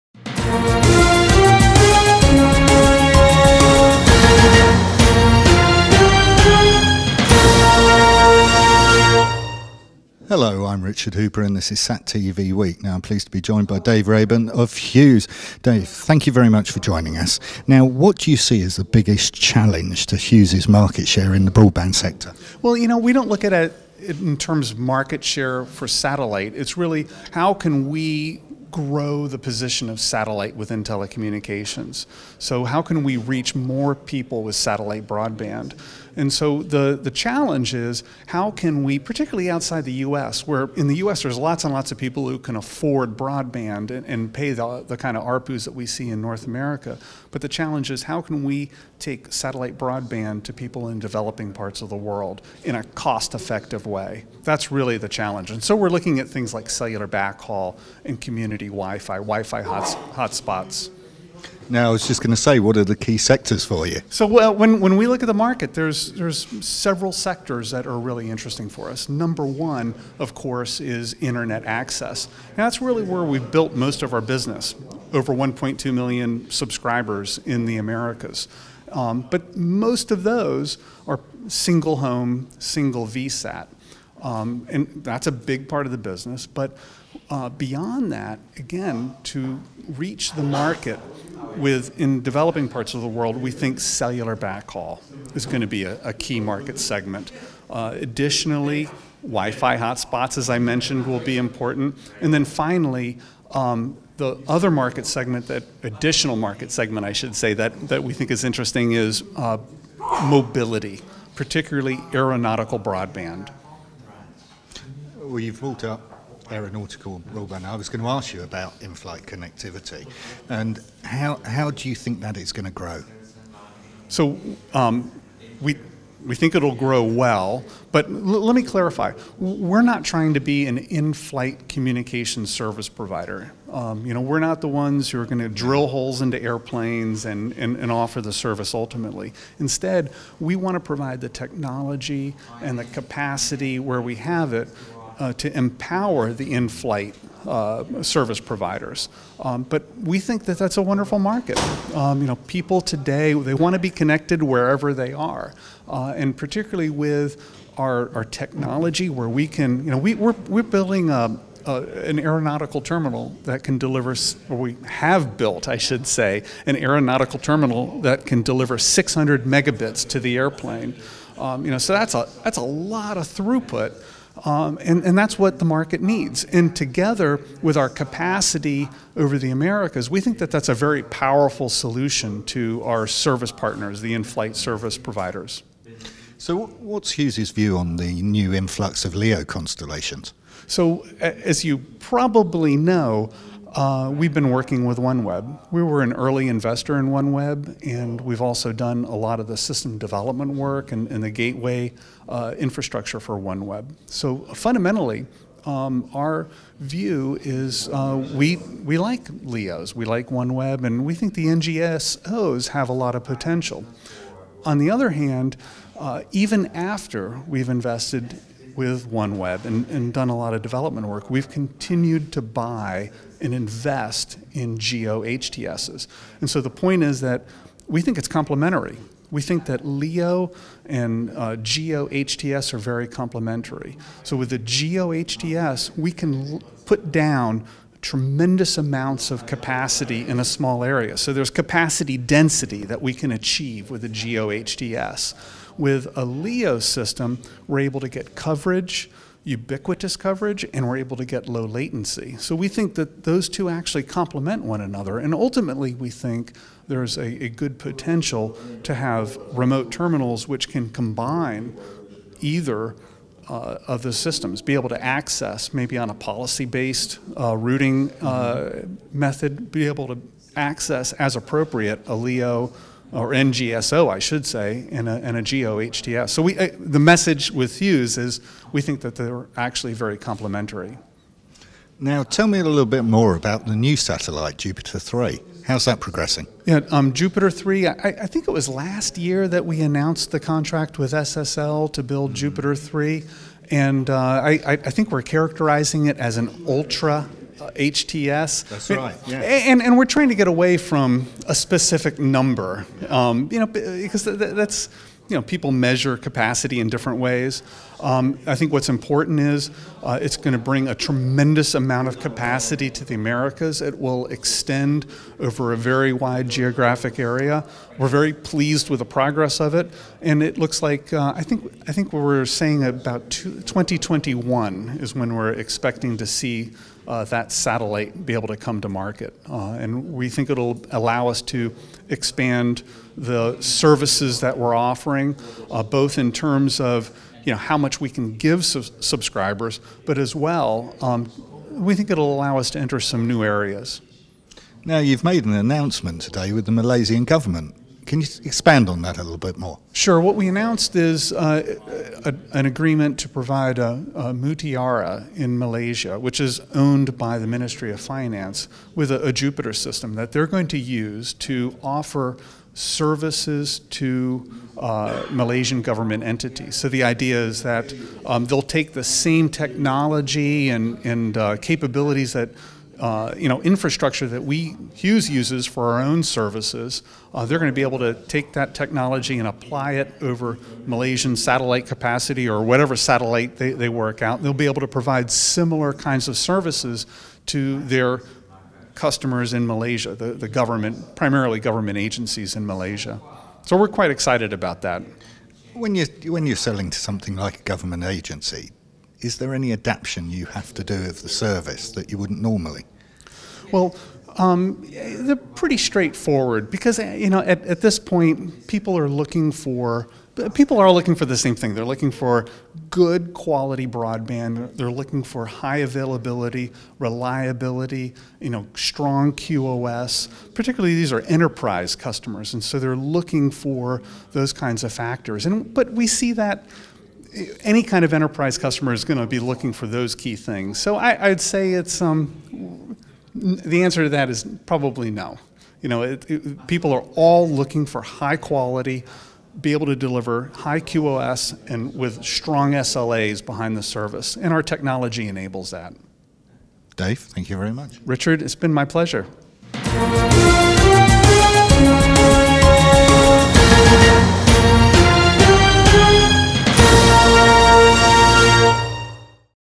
SatTV talks to Hughes Network Systems
Richard Hooper talks to Dave Rehbehn, VP International Division, Hughes about the company's broadband market share and its approach to cellular backhaul and inflight communications.